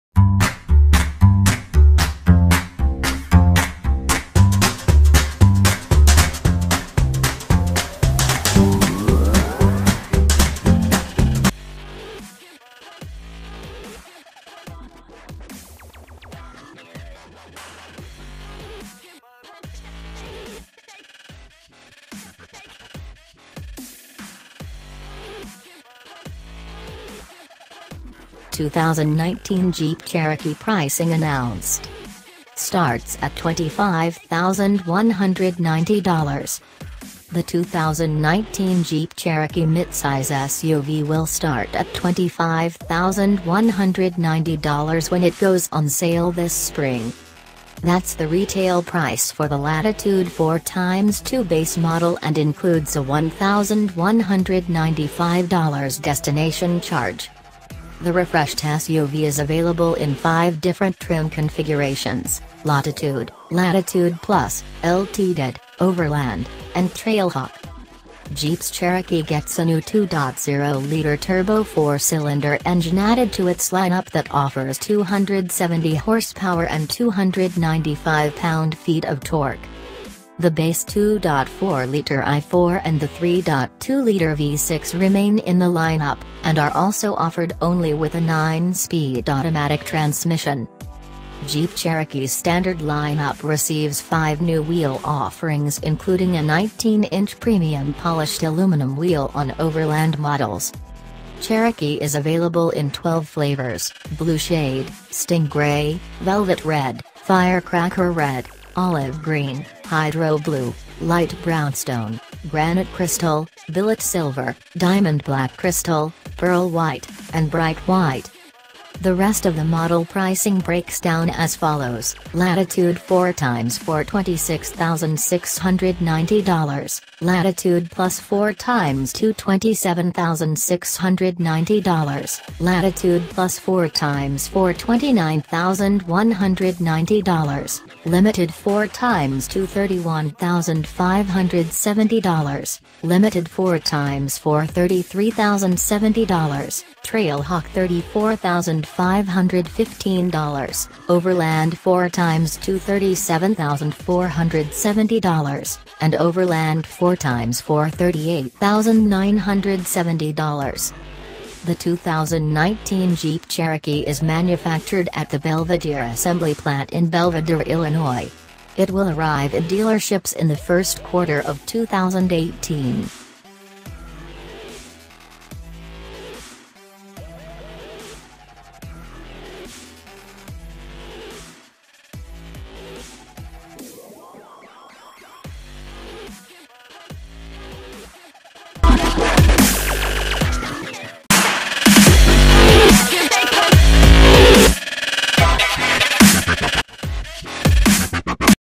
This is The Price Announcement Jeep Cherokee 2019
he 2019 Jeep Cherokee midsize SUV will start at $25,190 when it goes on sale this spring.

That’s the retail price for the Latitude 4×2 base model and includes a $1,195 destination charge.

The refreshed SUV is available in five different trim configurations: Latitude, Latitude Plus, Limited, Overland, and Trailhawk.

Jeep’s Cherokee gets a new 2.0-liter turbo four-cylinder engine added to its lineup that offers 270 horsepower and 295 pound-feet of torque.

1.

2.

3.